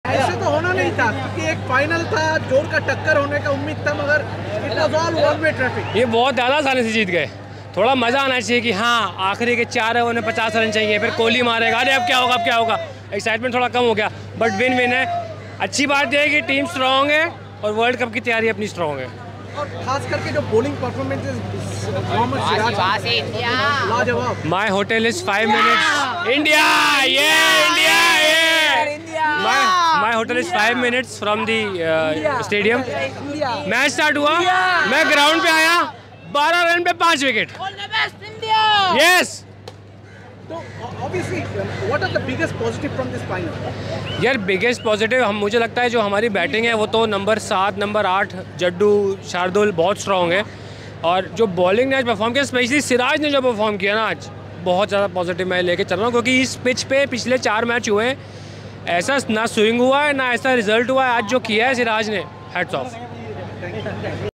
Famous youtuber Gaurav Taneja aka The Flying Beast reacts to India's Asia Cup triumph.
We can feel your pain Gaurav Taneja

It seemed like Rohit Sharma & his boys were in a hurry.